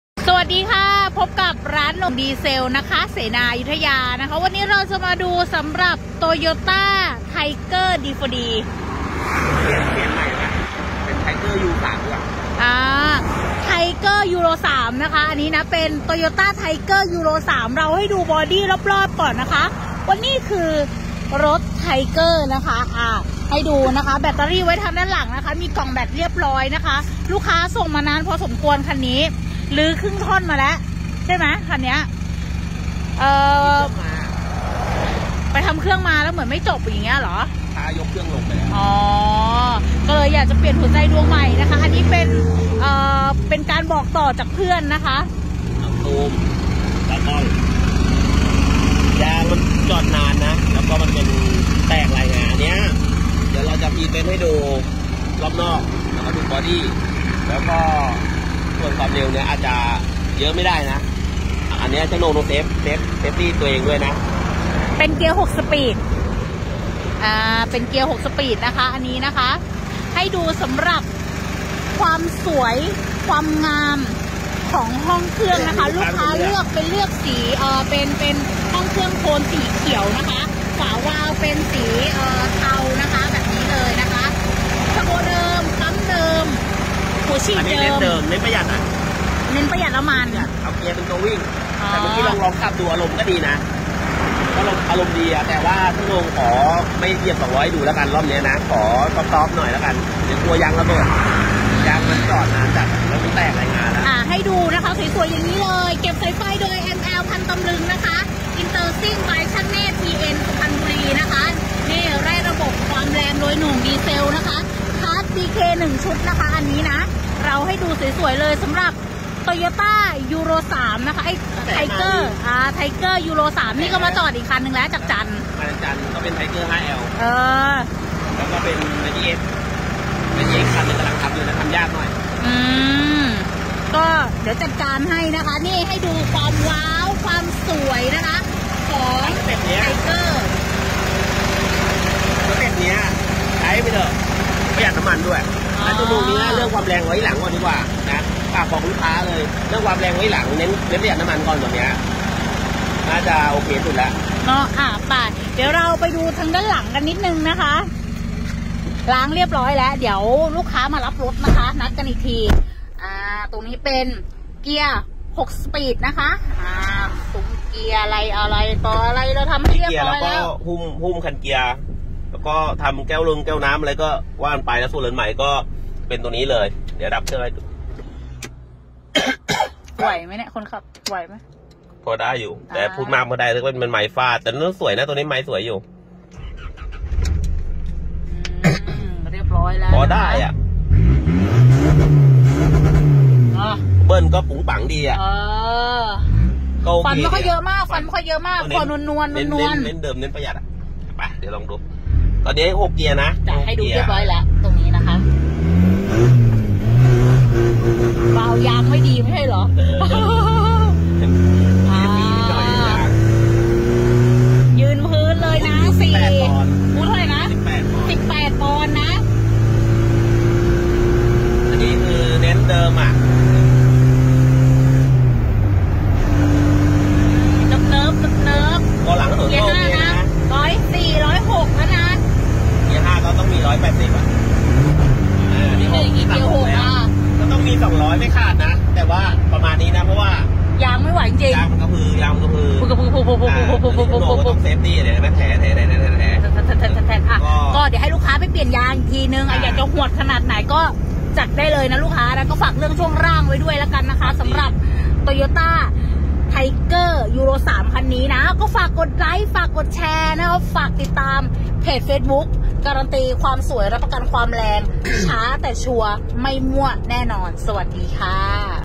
Tiger D4D ยูโร3 เทสรัน200 Km.วิ่งสบายๆ เน้นประหยัดน้ำมัน#โหน่งดีเซล เสนา อยุธยา-Pro Speed
Tiger D4D ยูโร 3!!! เทสรัน 200 Km.
วิ่งสบายๆ เทอร์โบ-เดิมหัวฉีด-เดิมปั๊มเดิม เน้นประหยัดน้ำมัน แรงดีดี๊ไม่มีทรยศ โหน่งดีเซล เสนา อยุธยา-Pro Speed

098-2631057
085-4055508